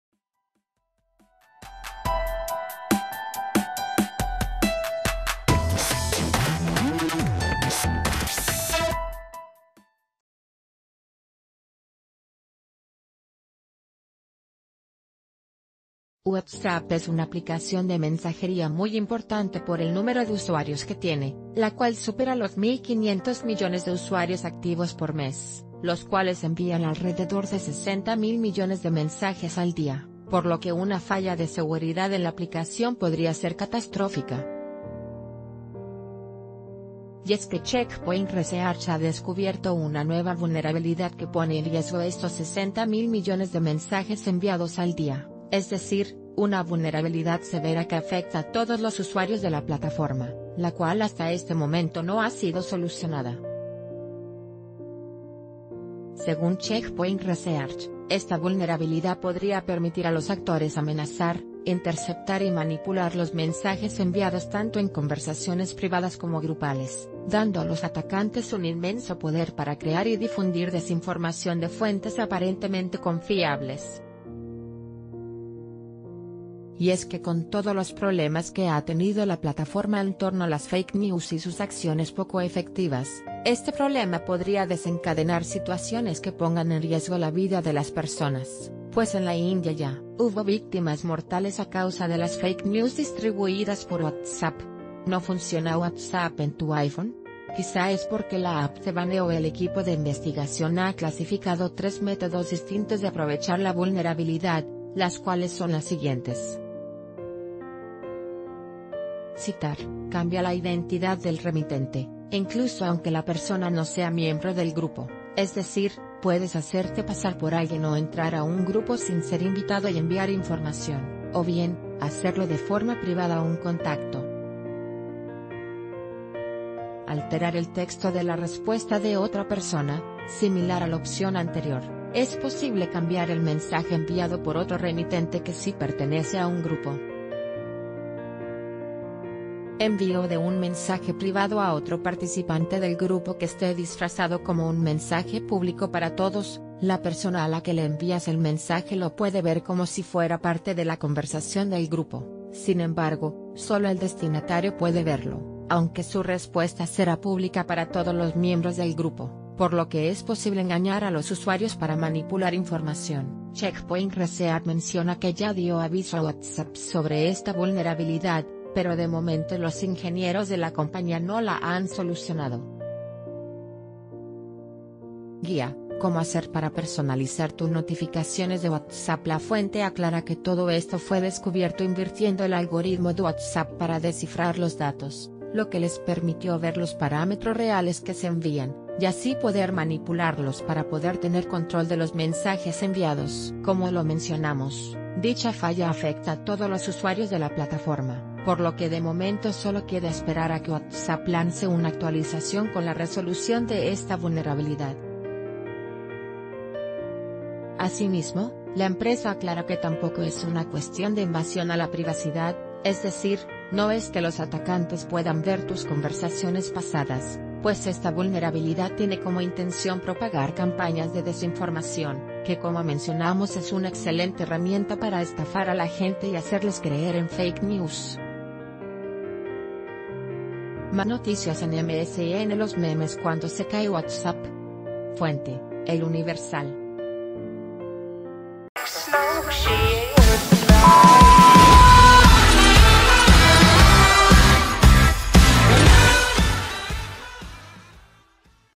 Nueva vulnerabilidad de WhatsApp pone en riesgo tus mensajes enviados
WhatsApp es una aplicación de mensajería muy importante por el número de usuarios que tiene, la cual supera los 1,500 millones de usuarios activos por mes, los cuales envían alrededor de 60,000 millones de mensajes al día, por lo que una falla de seguridad en la aplicación podría ser catastrófica. Y...